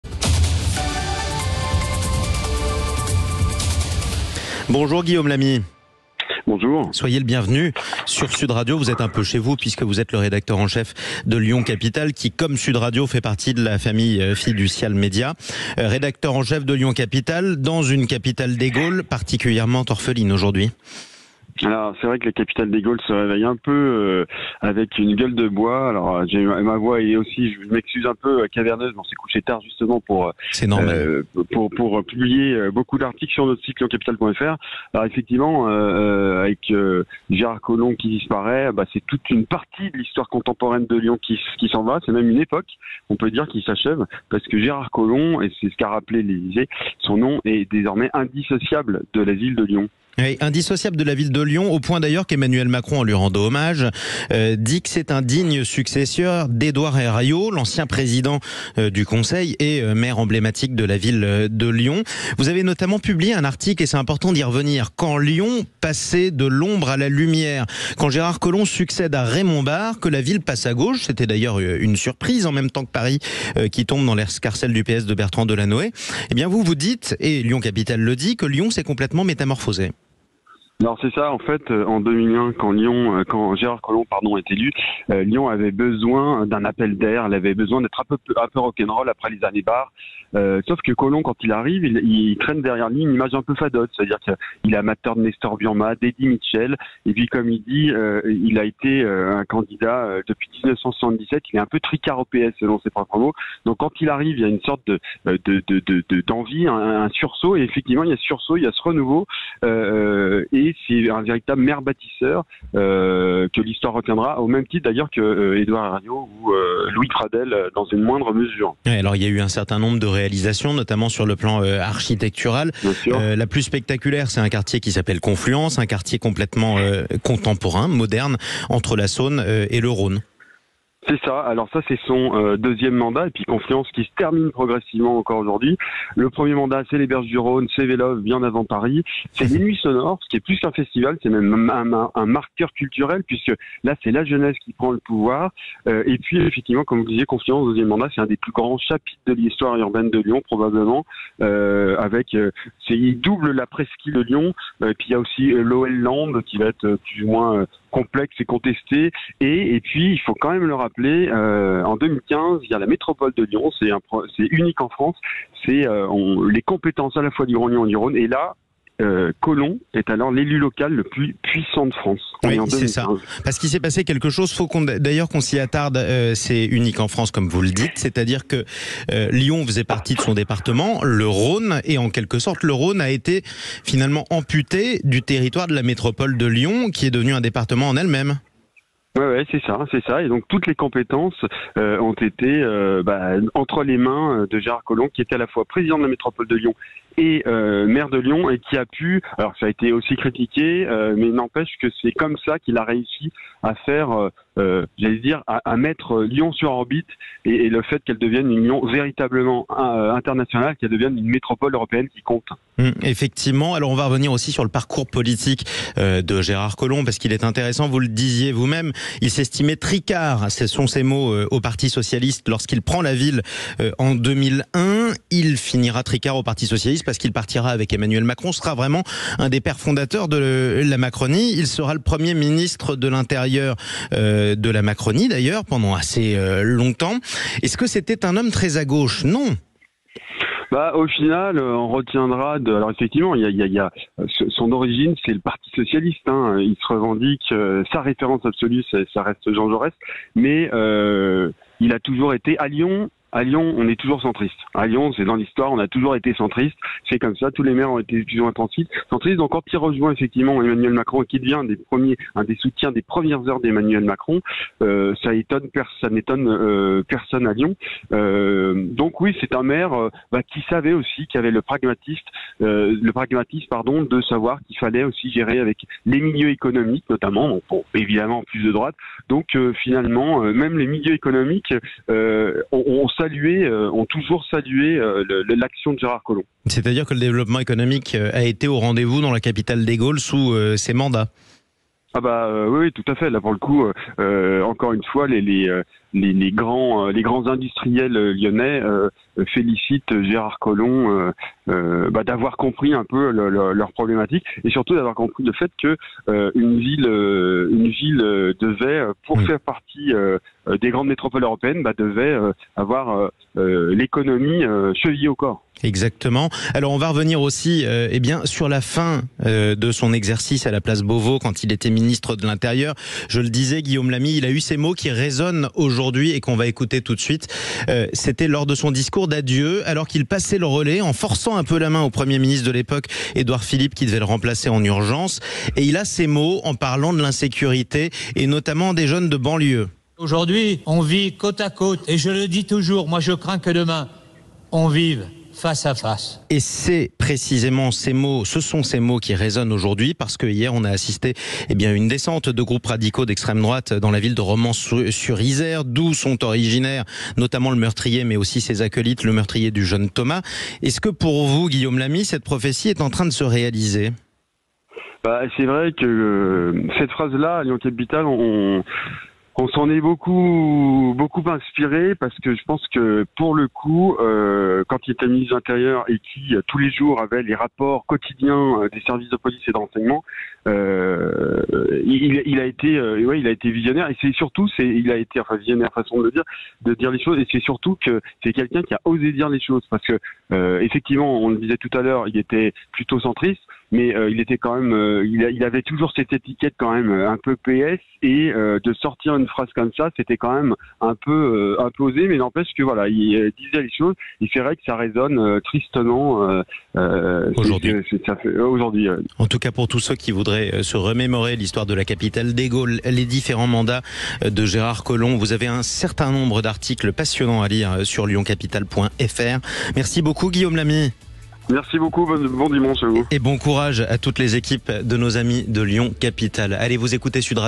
Mort de Gérard Collomb : les Lyonnais orphelins
Avec Guillaume Lamy, rédacteur en chef de Lyon Capitale.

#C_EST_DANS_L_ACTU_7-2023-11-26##